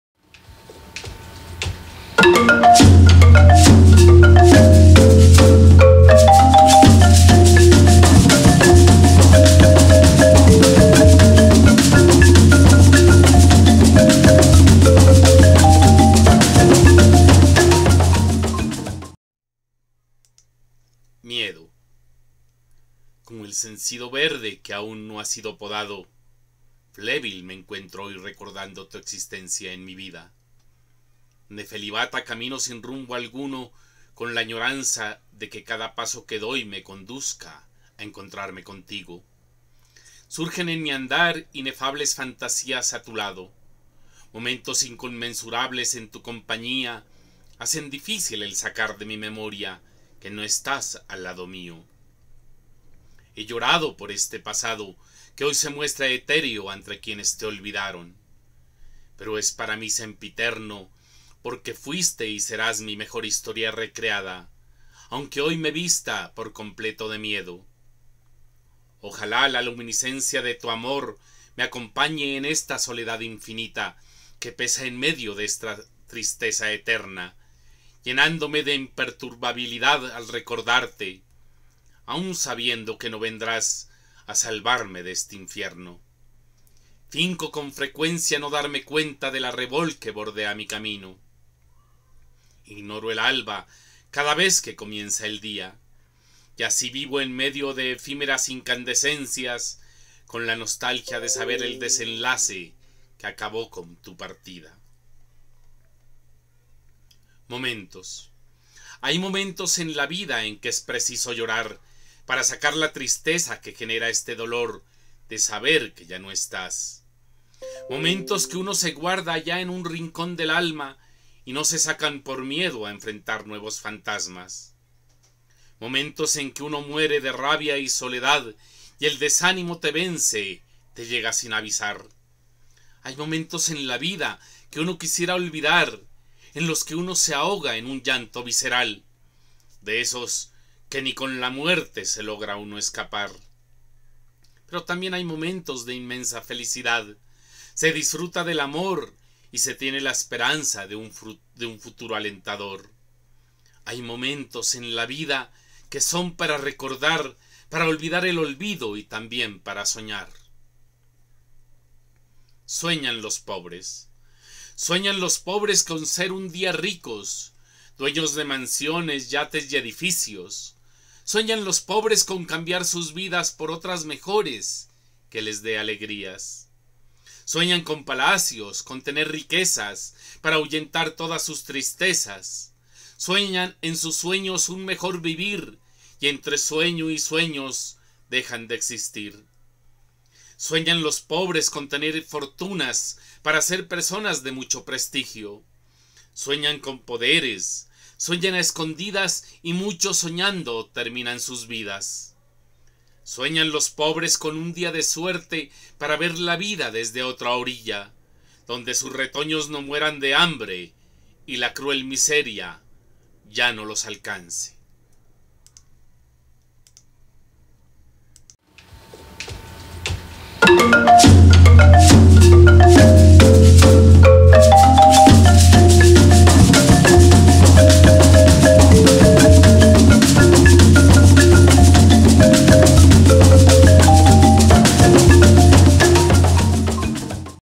Mauricio Chaves-Bustos lee Miedo, Momentos y Sueñan los pobres de Entre el olvido y la esperanza
Mauricio Chaves-Bustos, de Bogotá, lee “Miedo”, “Momentos” y “Sueñan los pobres” de “Entre el olvido y la esperanza” de Jenny Tenorio Caicedo.